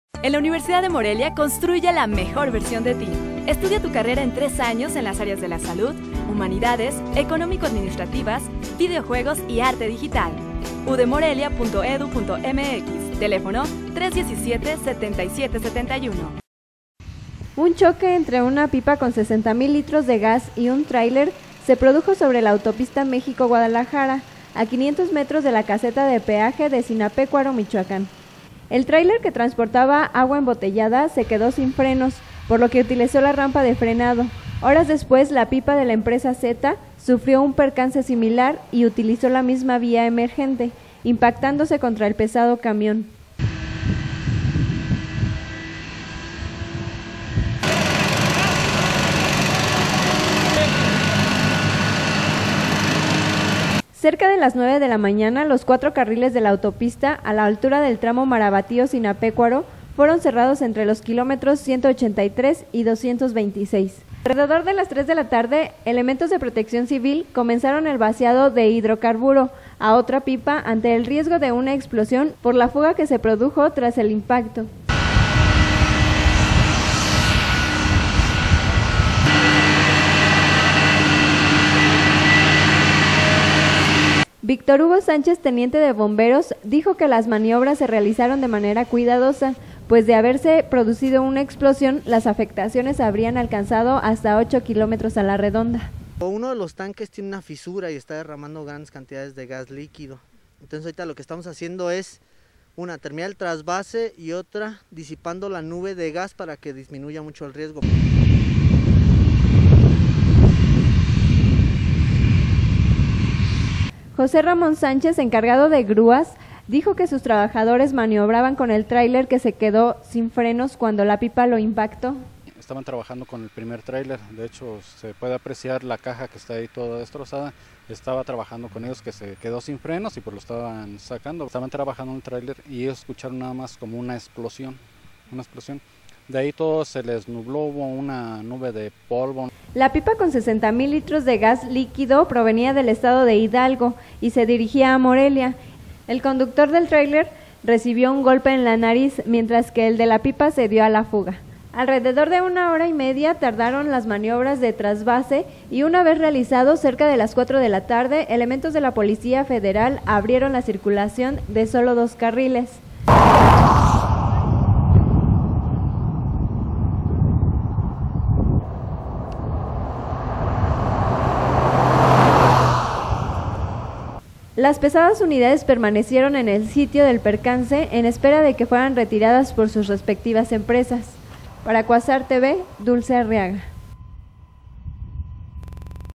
Choca pipa con 60 mil lts. de gas contra tráiler; PC evitó tragedia
Un choque entre una pipa con 60 mil litros de gas y un tráiler, se produjo sobre la autopista México-Guadalajara, a 500 metros de la caseta de peaje de Zinapécuaro, Michoacán...